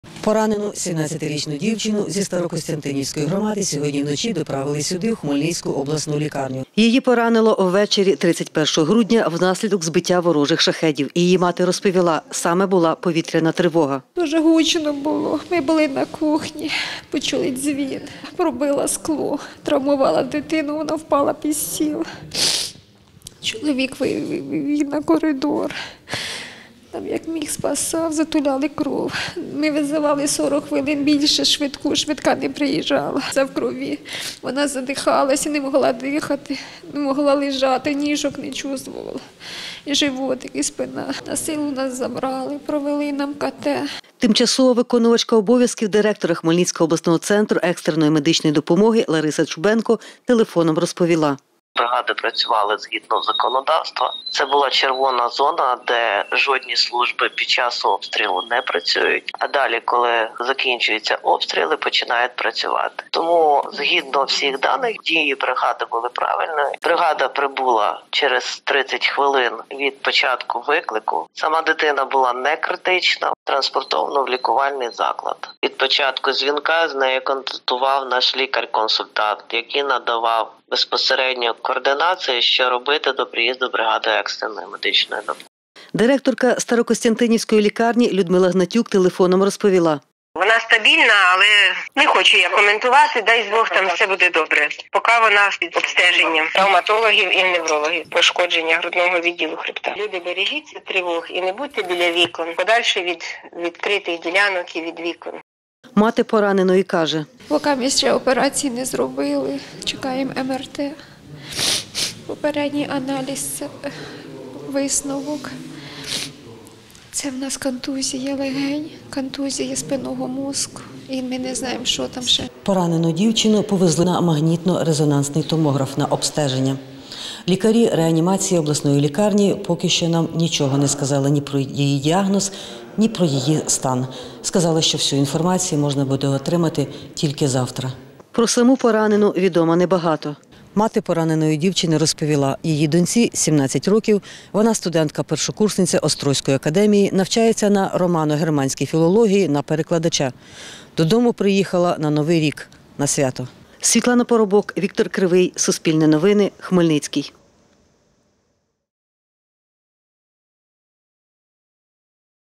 Дівчина отримала поранення внаслідок атаки "шахедів" на Хмельниччині
Внаслідок збиття ворожого безпілотника в Старокостянтинівській громаді поранено дівчину. Її батьки кажуть: їхня студентка-першокурсниця приїхала додому на свято, а опинилася в реанімації обласної лікарні.